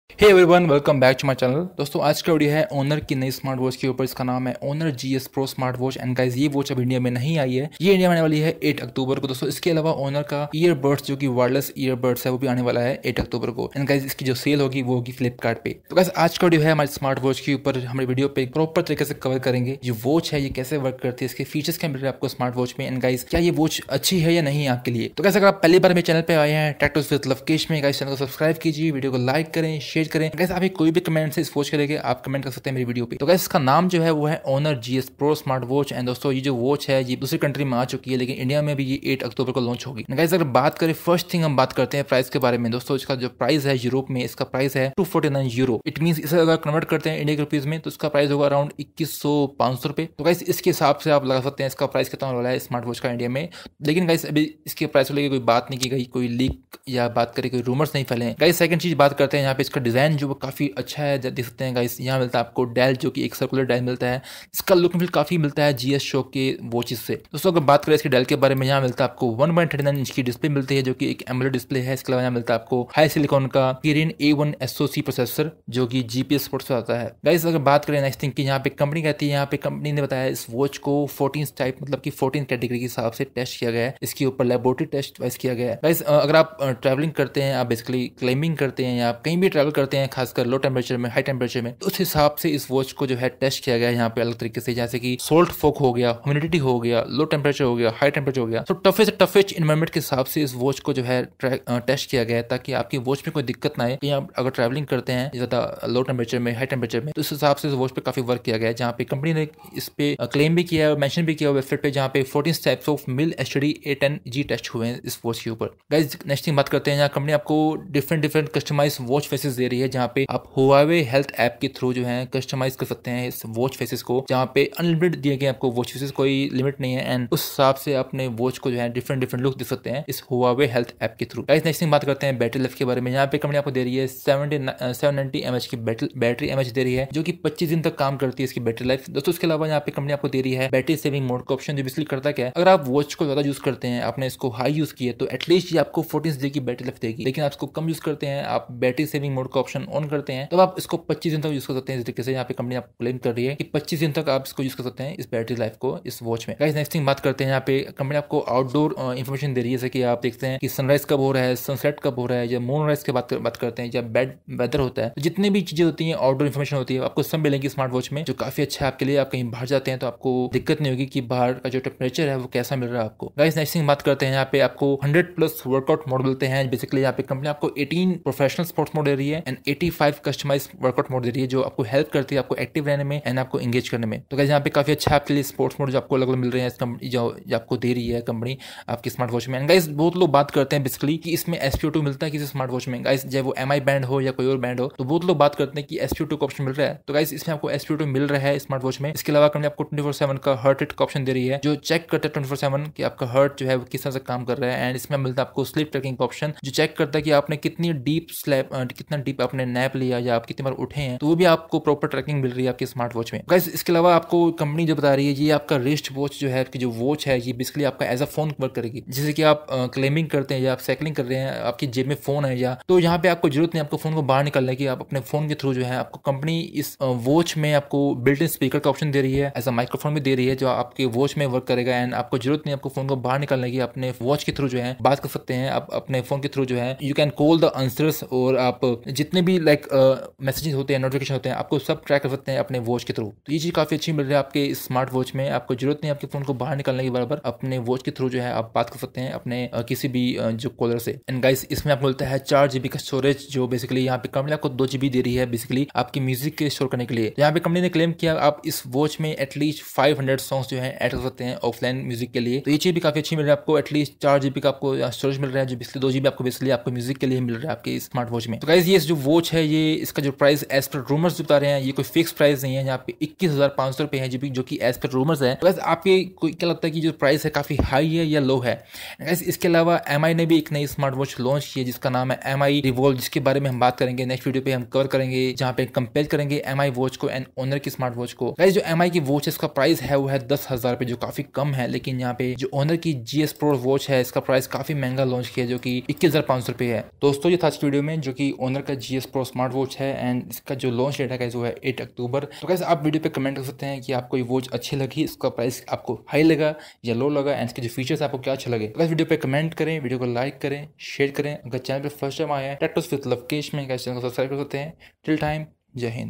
Honor GS Pro Watch | Full Specifications and Price | Honor Smartwatch with Spo2
Honor GS Pro smart watch features and price in india. All details in this video. Honor gs Pro wrist smart watch going to launch in india on 8 October. checkout full details in this video

"FOLLOW ME"